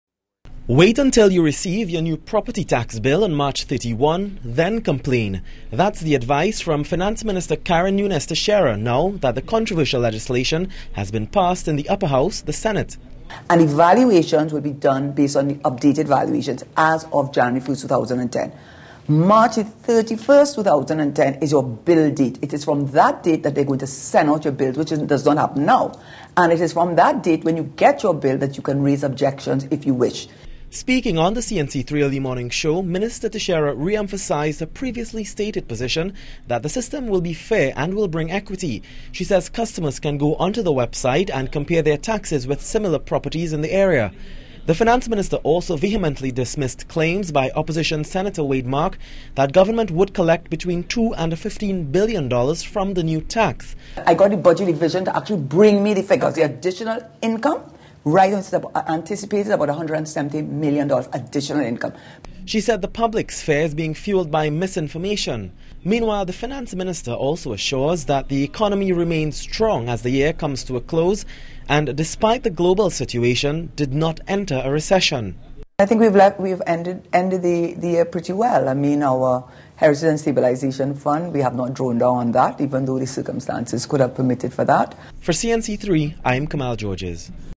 Minister says complain after property tax bills are received.flv
7pm news - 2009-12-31 - Minister says complain after property tax bills are received